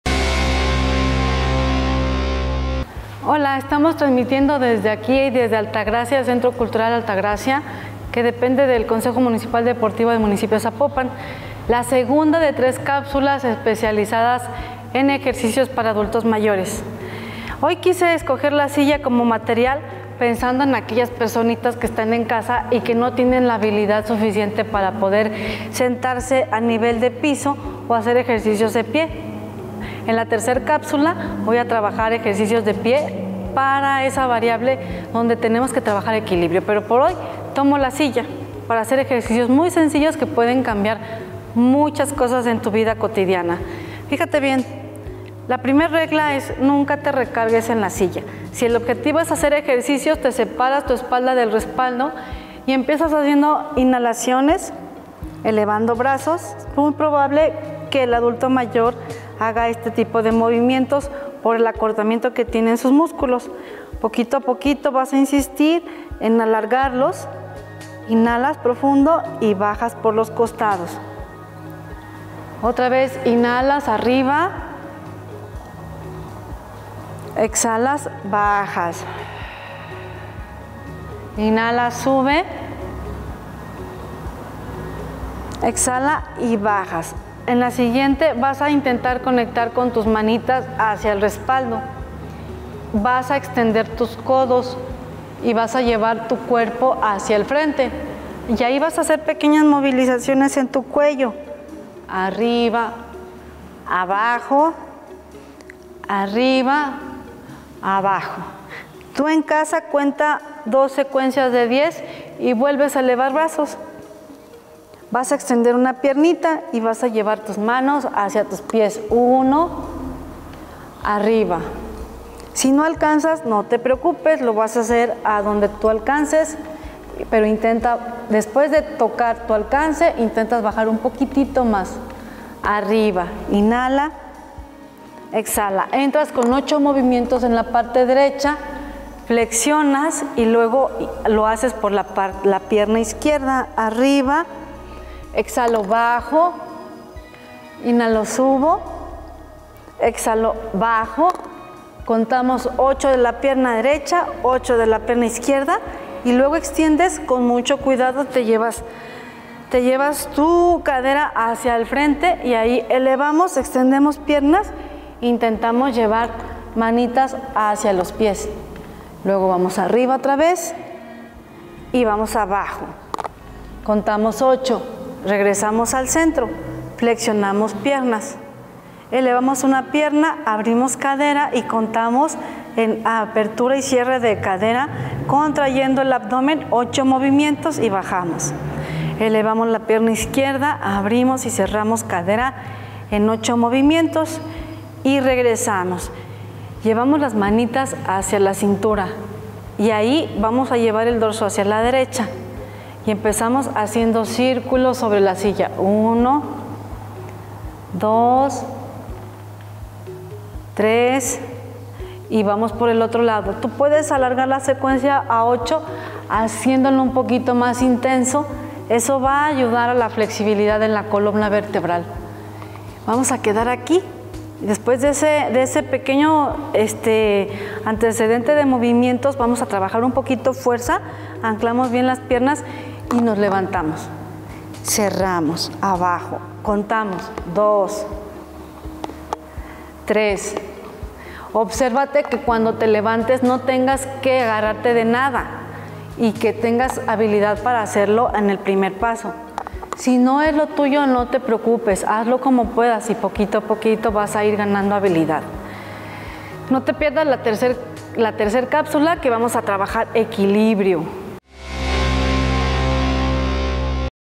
Activación Yoga con Silla
La instructora de Yoga Adriana Salazar utiliza, en esta cápsula, una silla con el fin de que personas que no puedan permanecer de pie dengan la posibilidad de ejercitar su cuerpo